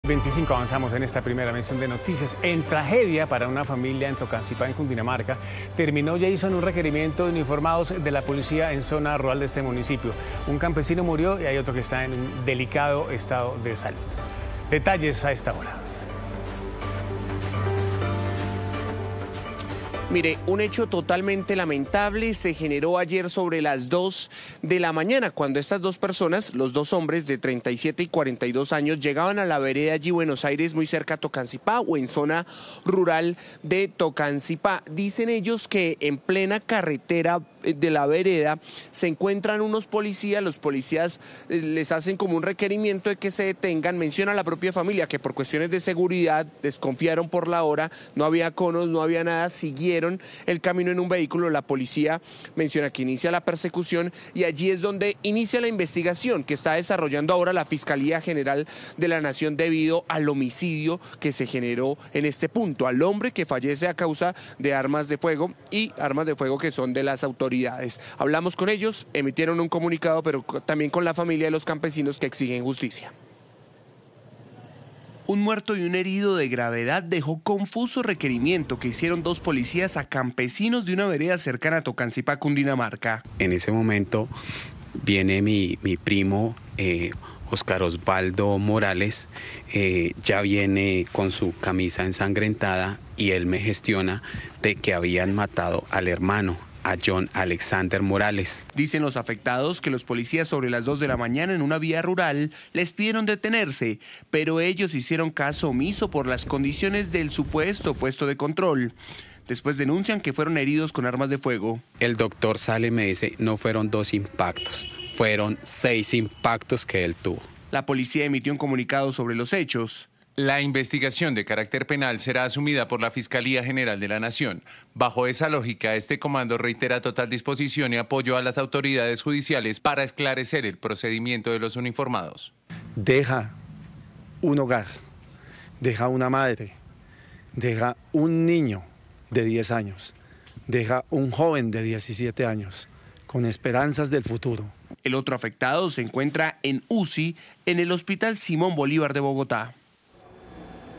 Autoridades investigan presunto caso de abuso de autoridad en Tocancipá
NoticiasRCN | En tragedia para una familia en Tocancipá, Cundinamarca, terminó un requerimiento de uniformados de la Policía en zona rural de este municipio. Un campesino murió y otro está e delicado estado de salud en un centro médico en Bogotá. Al parecer, los uniformados habrían disparado contra los ciudadanos.